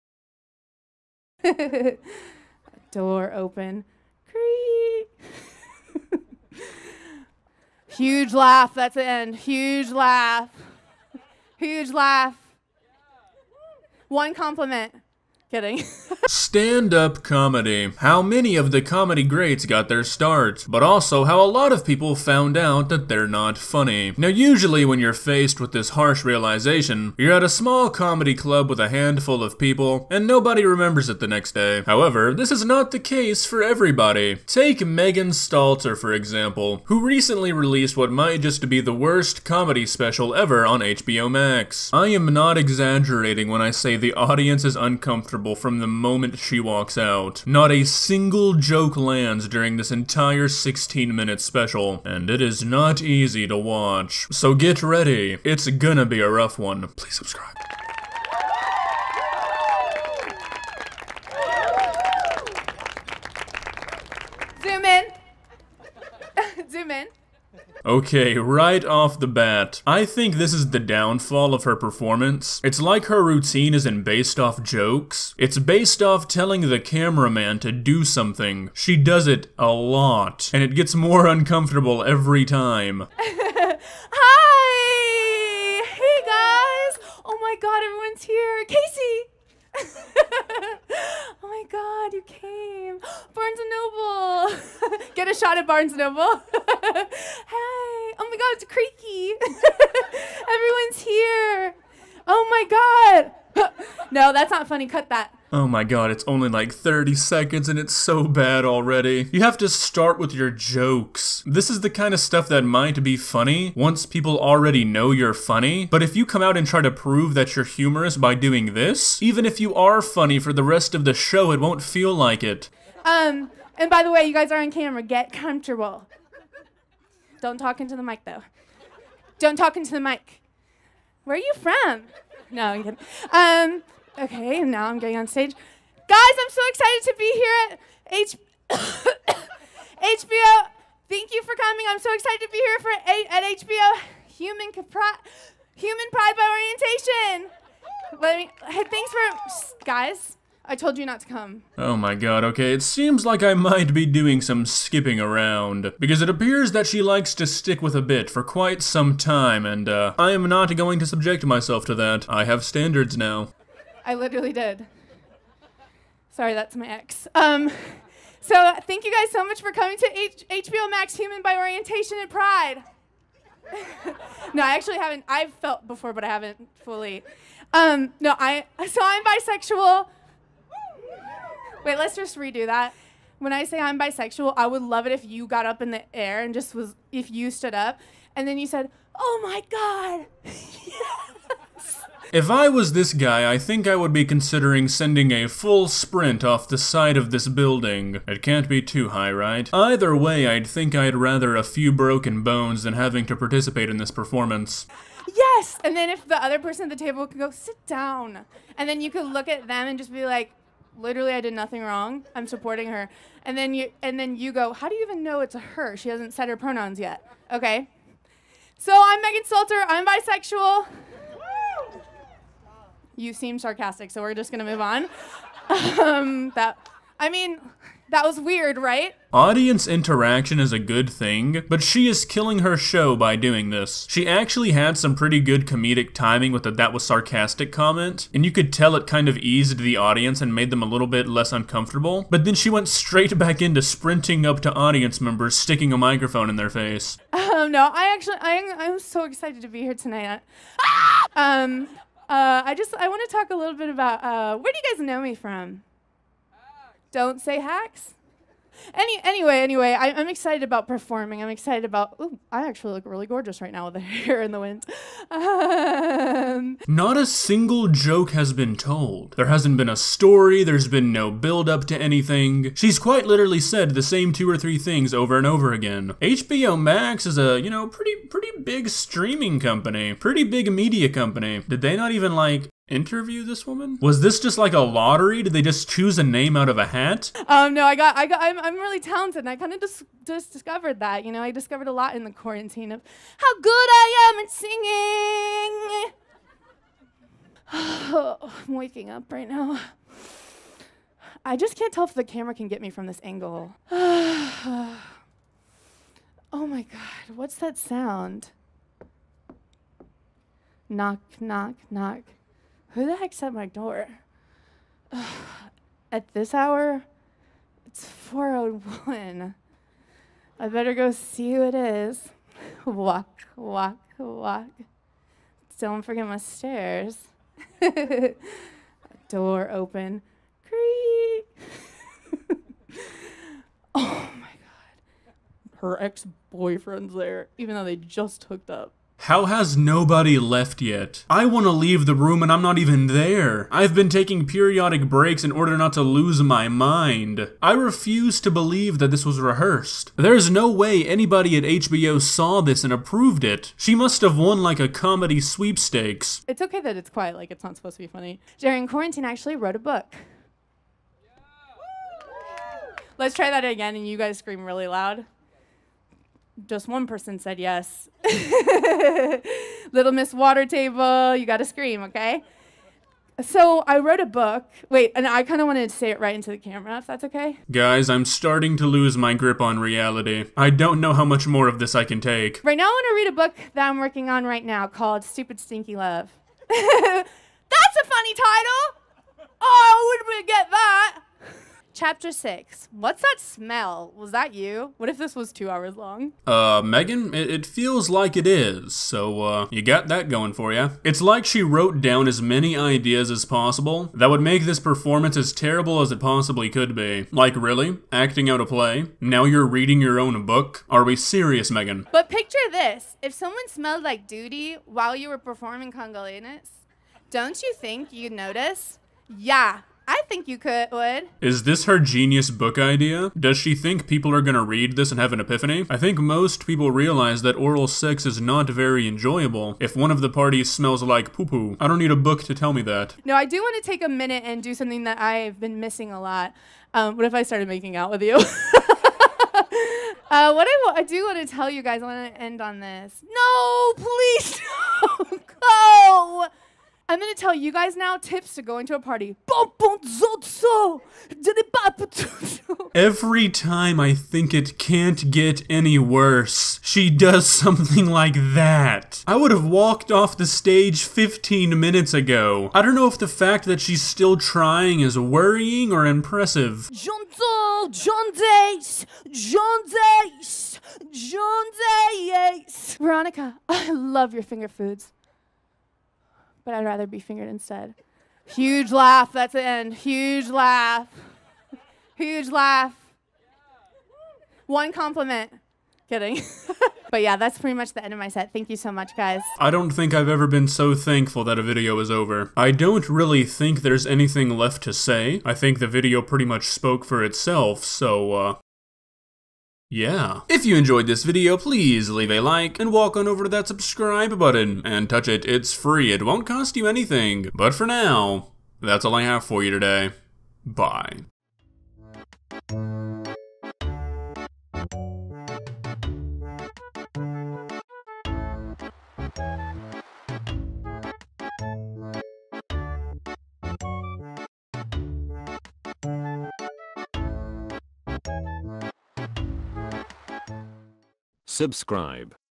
The Worst Stand Up Comedy in History
Recently, Megan Stalter was featured on an HBO Max comedy special, and it was without a doubt the worst stand up comedy performance in history. Prepare to cringe, a lot.